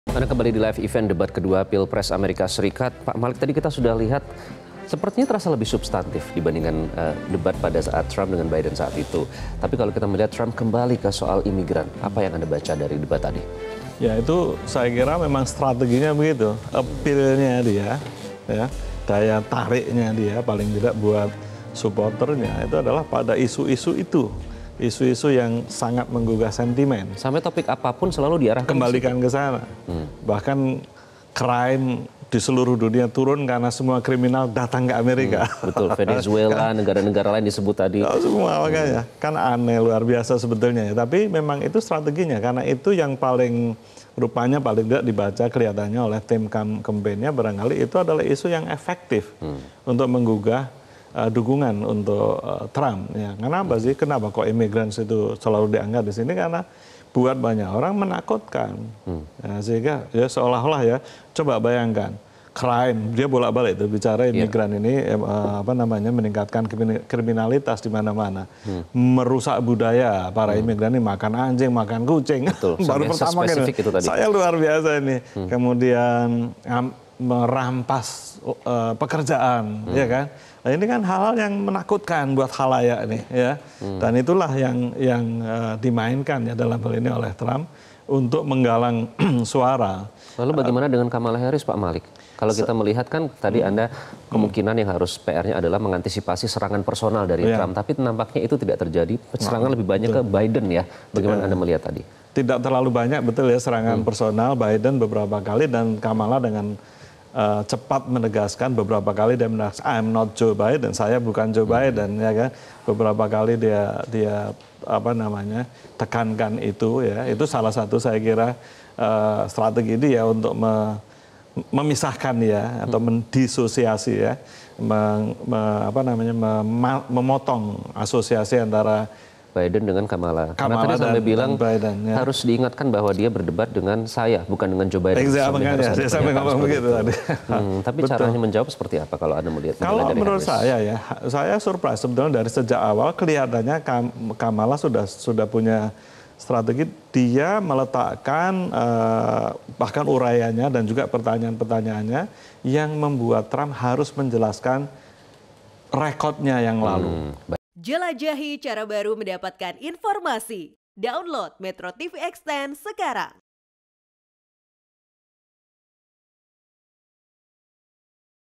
Trump Angkat Isu Terkait Imigran di Debat Pilpres
MetroTV, Calon Presiden (Capres) Partai Demokrat Kamala Harris dan Capres Partai Republik Donald Trump akan berhadapan dalam debat presiden pada Selasa, 10 September 2024, malam. Debat ini menjadi momen menentukan bagi Pemilu Amerika Serikat (AS) di November mendatang.